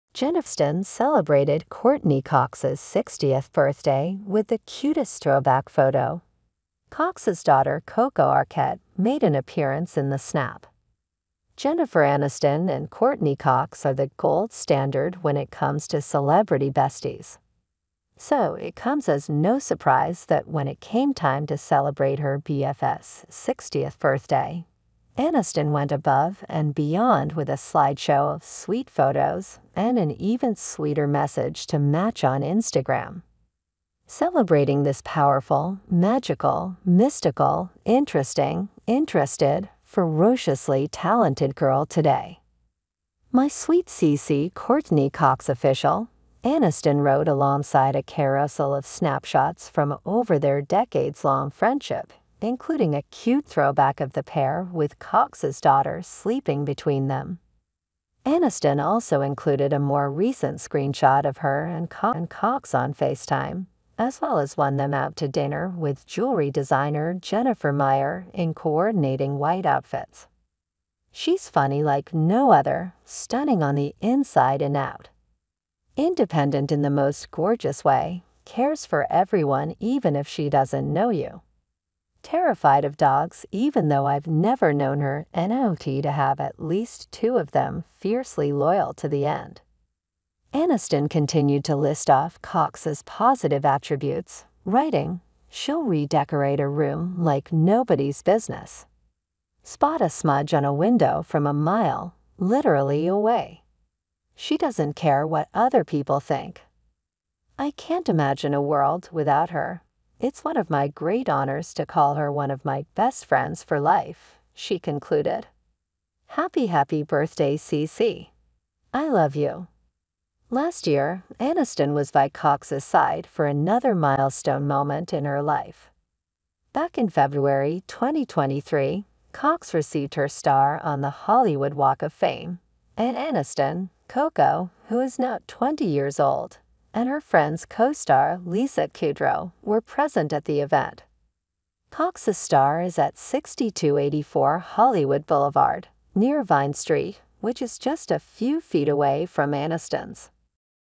Jennifer Aniston Celebrated Courteney Cox's 60th Birthday With the Cutest Throwback Photo
Jennifer Aniston celebrated Courteney Cox's 60th birthday with a slideshow of sweet photos and a message on Instagram. Aniston shared a carousel of snapshots from their decades-long friendship, including a cute throwback of the pair with Cox's daughter Coco Arquette sleeping between them. She also included a recent screenshot of her and Cox on FaceTime and one out to dinner with jewelry designer Jennifer Meyer in coordinating white outfits. Aniston praised Cox's positive attributes, including her ability to redecorate a room and her loyalty to her friends. She concluded by saying she couldn't imagine a world without her.